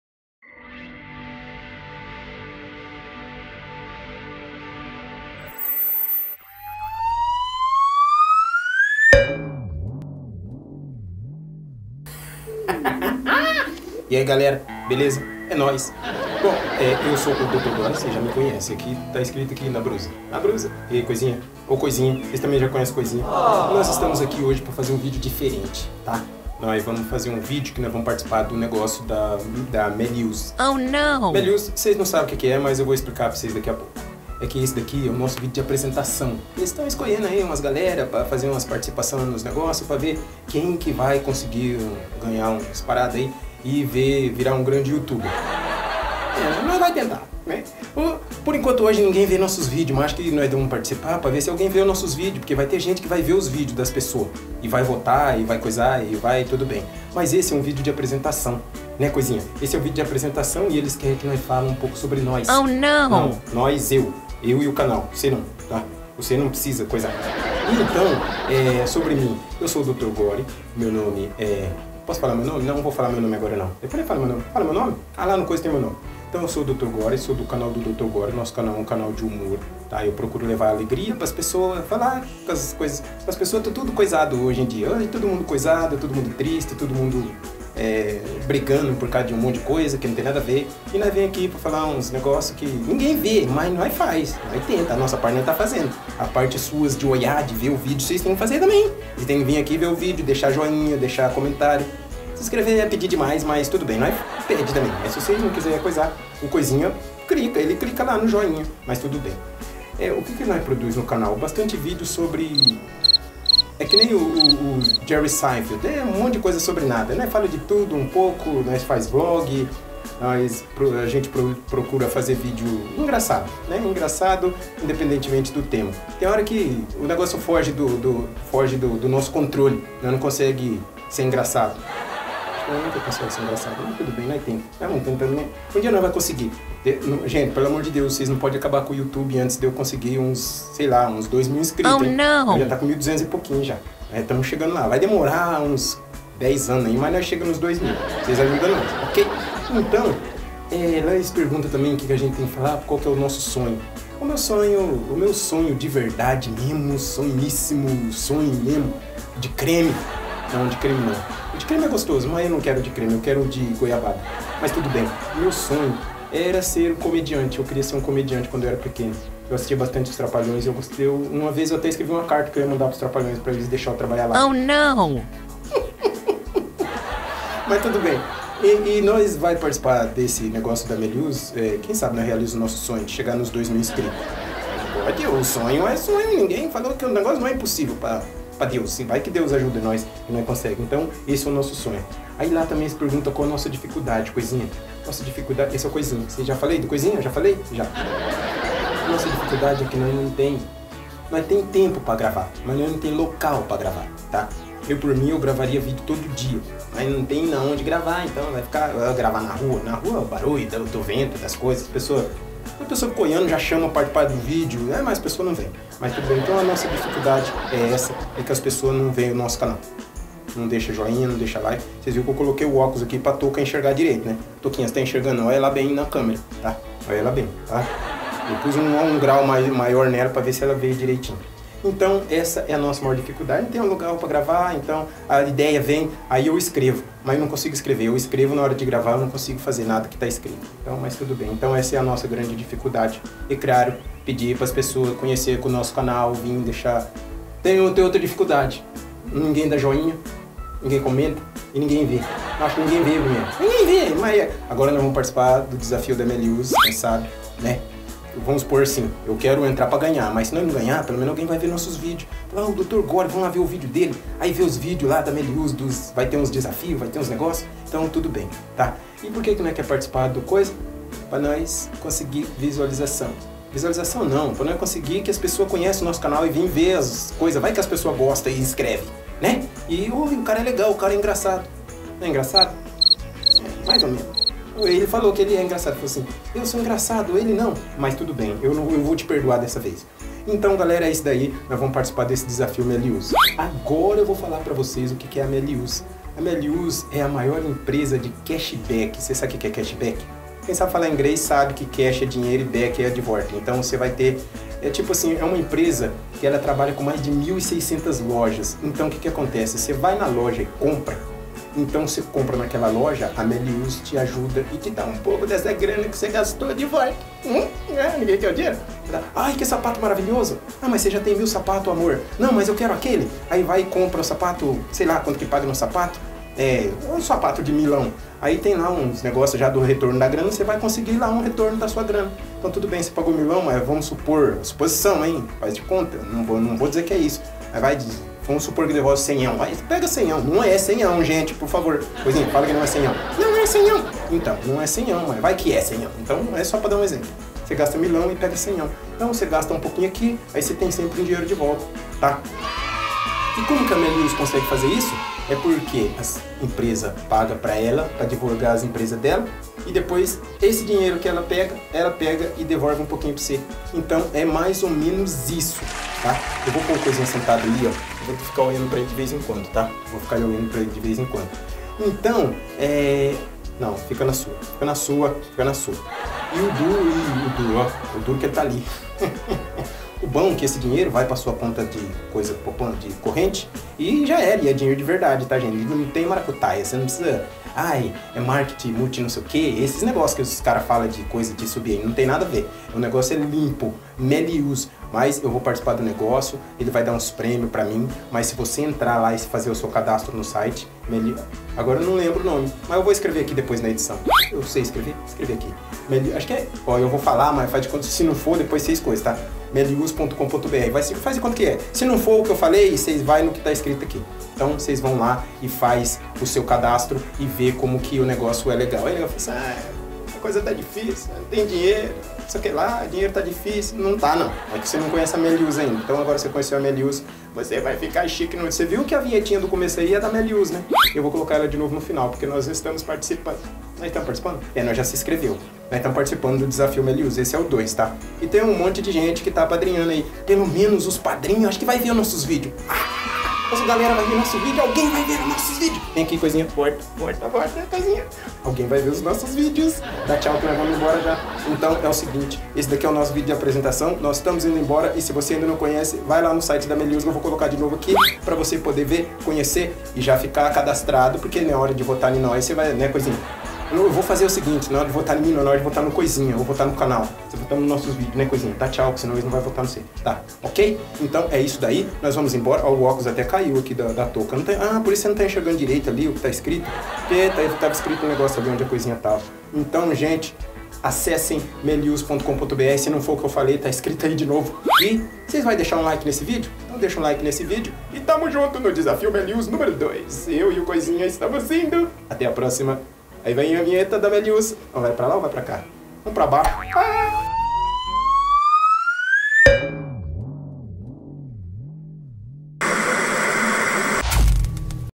Desafio Méliuz #2 - Apresentação
Este é o meu vídeo de apresentação do Desafio Méliuz #2 2018.
Não conhece a Méliuz vejam vídeo e confira o que é o Desafio Méliuz 2018 essa é a segunda edição espero ter uma boa participação.
Acesse o site e conheça a Méliuz , o endereço está no vídeo.
Nesse vídeo temos a participação especial do nosso amigo Koizinha , espero que ele não tenha estragado minha representação kkkk
Esse é apenas o começo espero poder contar com a ajuda de todos vocês Ok? Ok? Ok?